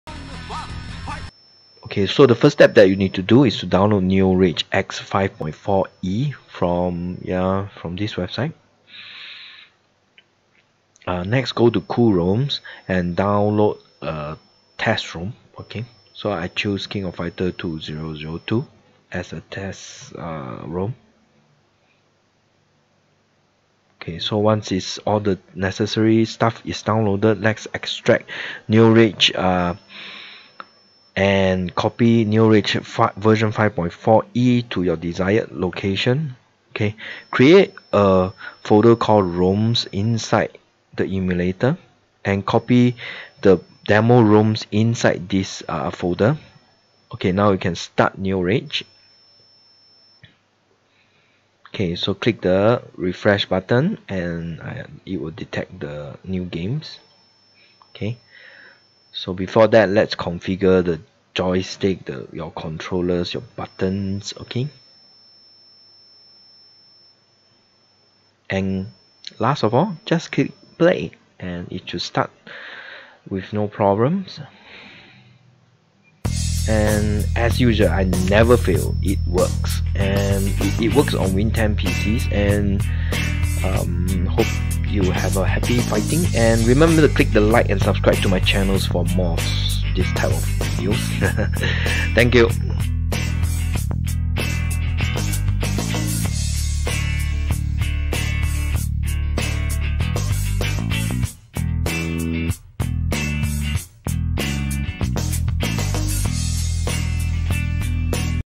NeoRAGEx 5.0 Free Download 2021 for MS Win Free
Password : 2020

Hello guys
Today in this video I will show you how to download neogeo emulator (NeoRAGESx 5.0) and play best neogeo game on window must watch.